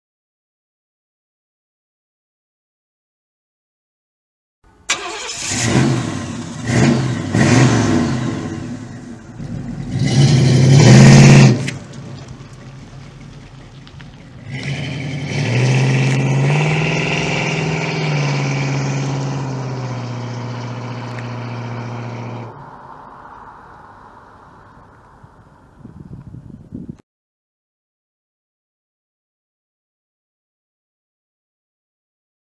Pontiac LeMans, American muscle sounds! Full HD
Short video after my photoshoot for Buena Vida Autoverhuur.